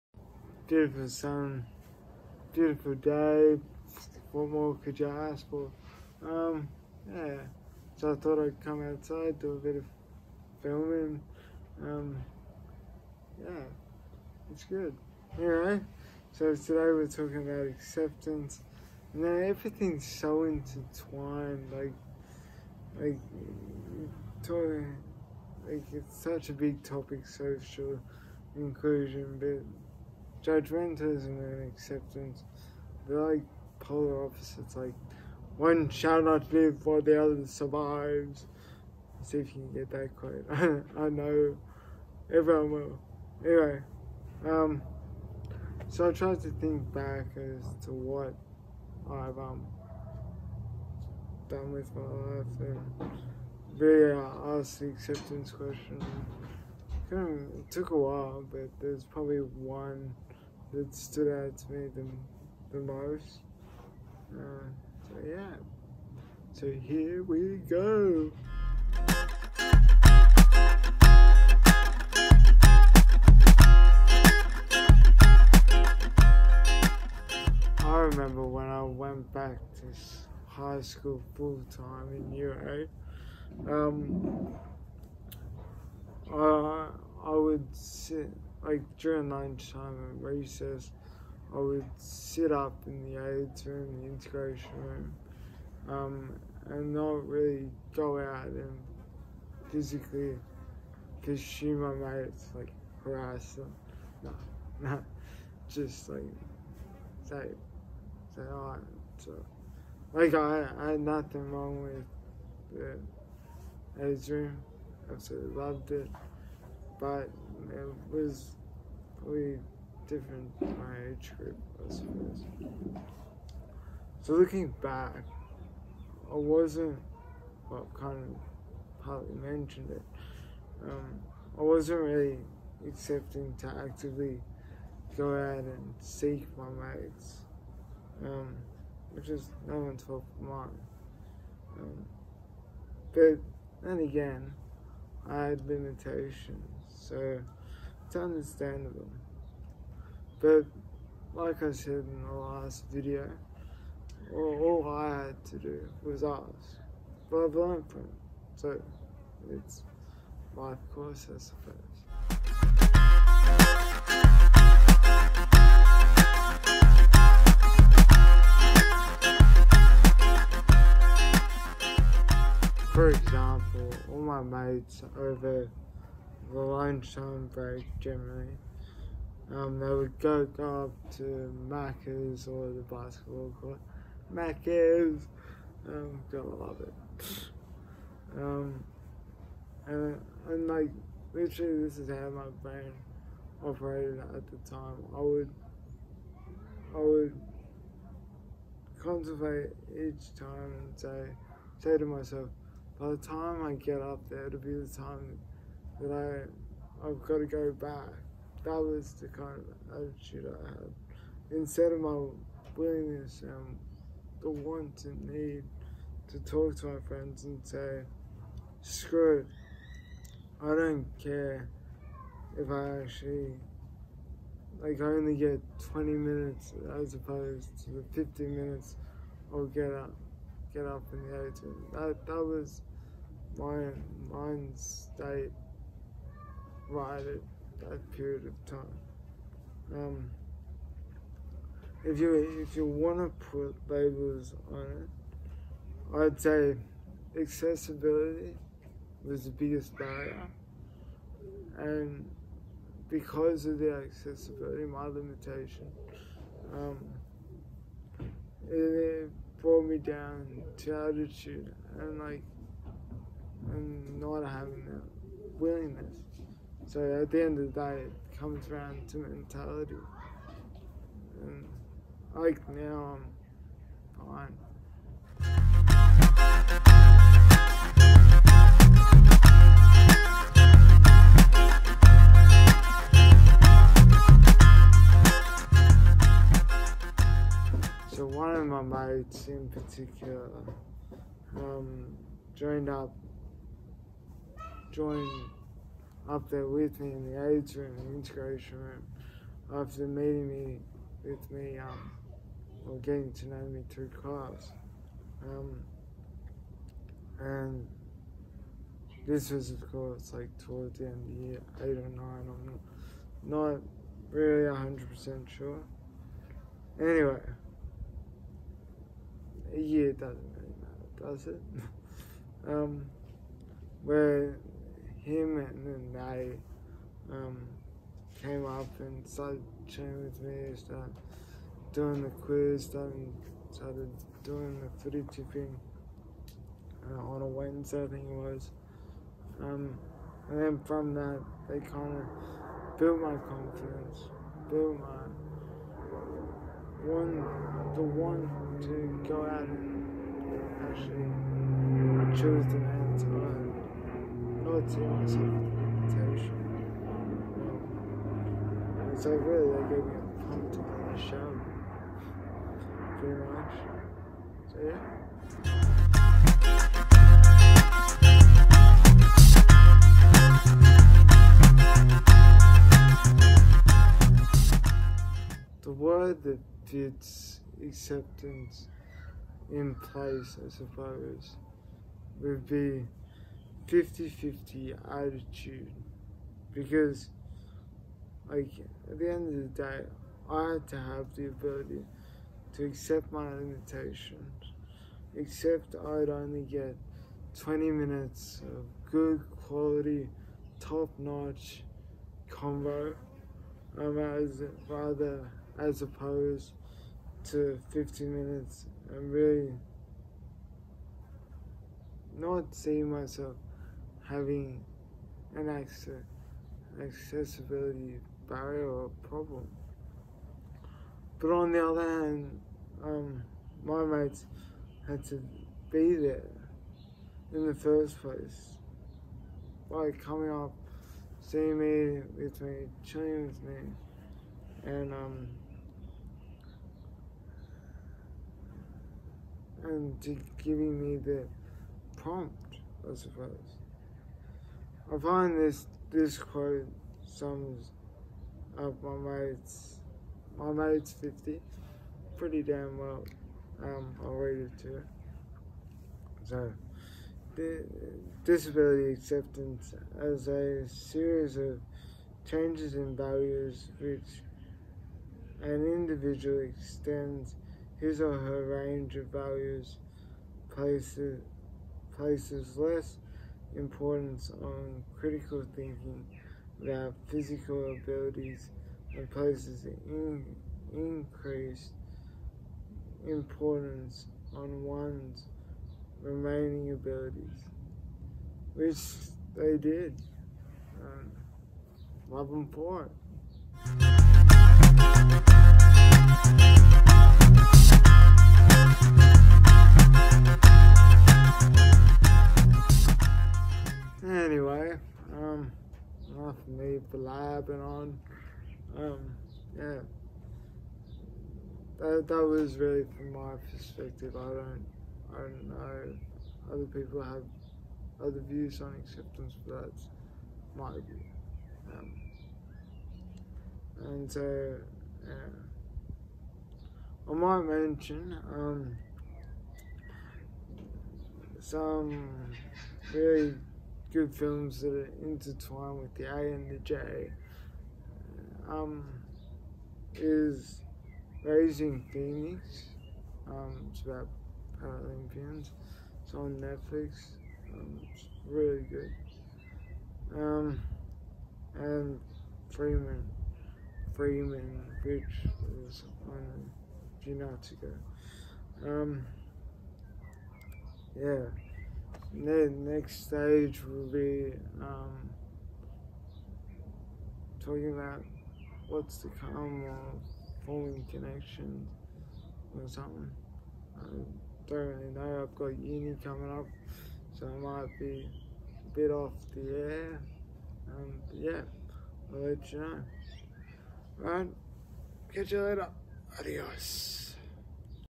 Acceptance and my barriers
This Ep I'll be talking about how I overcame my acceptance that was weighing me down with a little bit of help.